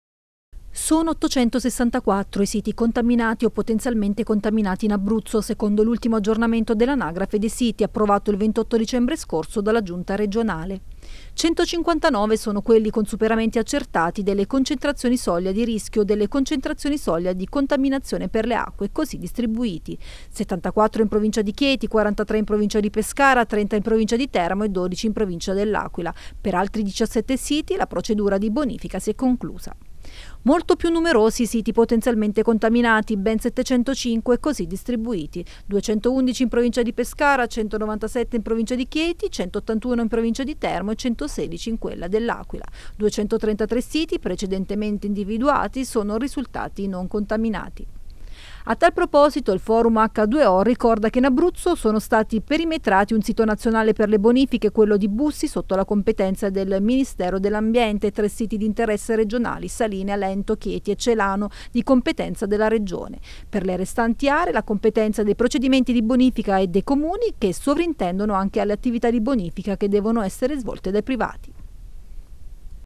In Abruzzo 864 siti contaminati
In seguito alla mappa dei siti inquinati stilata dalla Regione, il Forum H2O lancia un grido di allarme e chiede al governo che si insedierà dopo le elezioni del 10 febbraio di uno sforzo specifico su questo tema